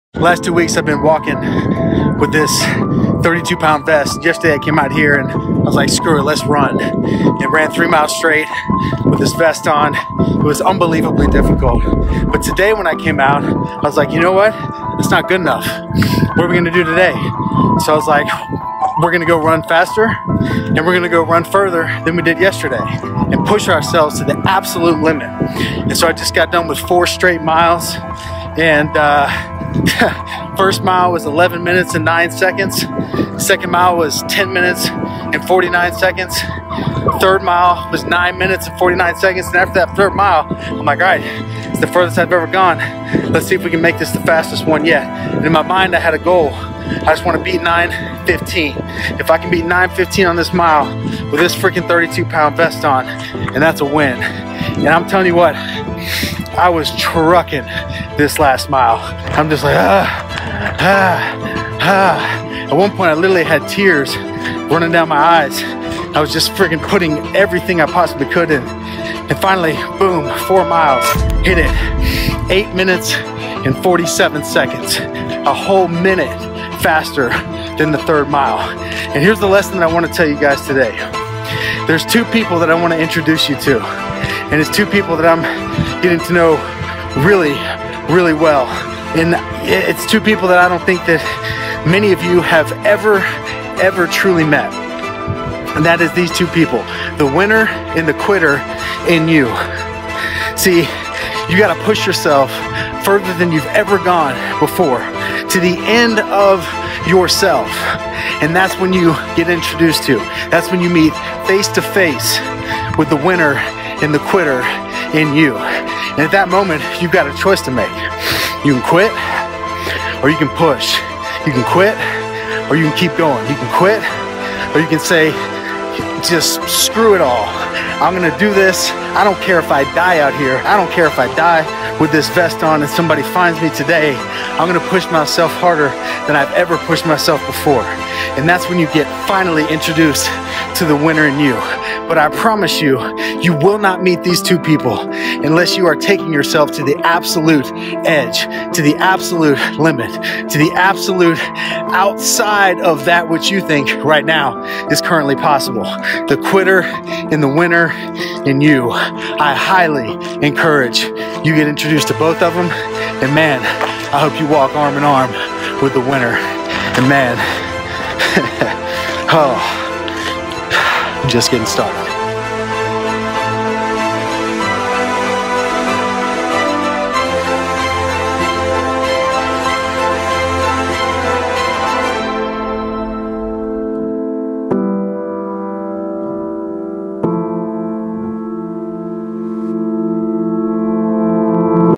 When the Quitter Meets the Winner in You | My Living Legacy | Ep. 75
Have you met these two people within you yet?

If you haven't had the chance to experience their presence in a powerful way, then it's time to push yourself to the ABSOLUTE LIMIT.

Your character in this life is directly reflected by the actions you choose to take.

So the question arises, which version of you do you desire to be?

It's time that you start taking the actions that move you towards the only "you" the world needs— the best version of yourself.